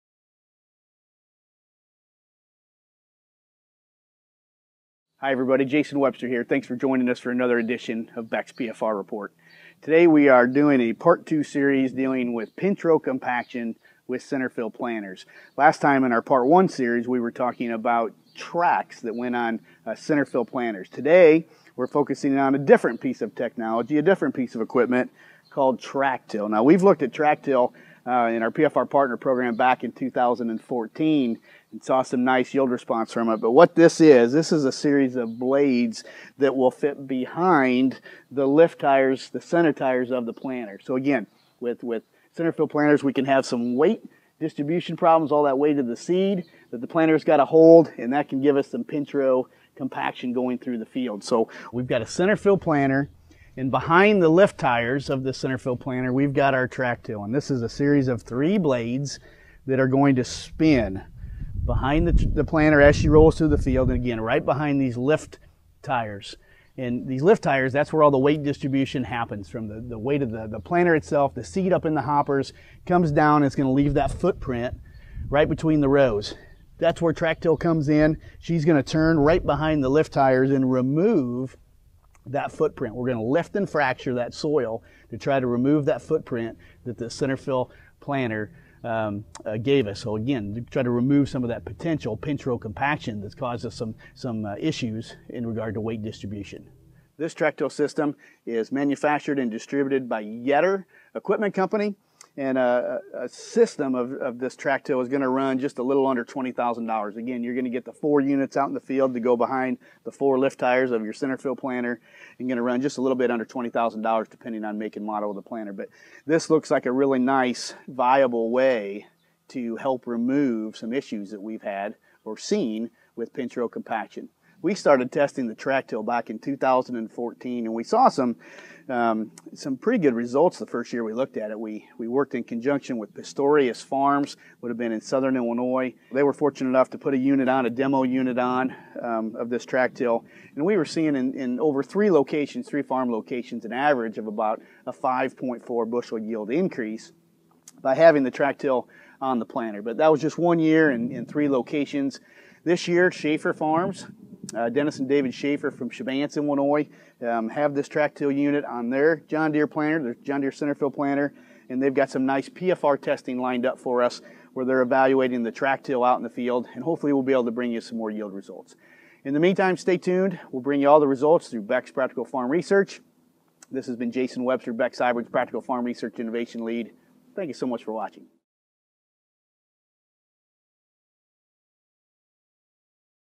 Beck’s PFR Report | TrackTill® | Pinch Row Compaction
Beck’s PFR Report | TrackTill® | What are the viable ways to combat pinch row compaction? Beck’s Practical Farm Research (PFR)® Innovation Lead, Jason Webster, introduces an on-farm study where we will attempt to combat pinch row compaction with the use of TrackTill. This planter attachment, manufactured by Yetter, is a series of blades that fit behind the center lift tires of the planter. TrackTill lifts and fractures the soil in an effort to eliminate wheel track, or central-fill pinch row compaction. Pinch row compaction leads to shorter, stunted plants, restricted roots and ultimately yield loss.

In 2014, PFR saw an average of 5.4 Bu./A. advantage with the use of TrackTill at three of our locations. We hope to bring you more yield results this season as we continue to test TrackTill. Check out the video to learn more.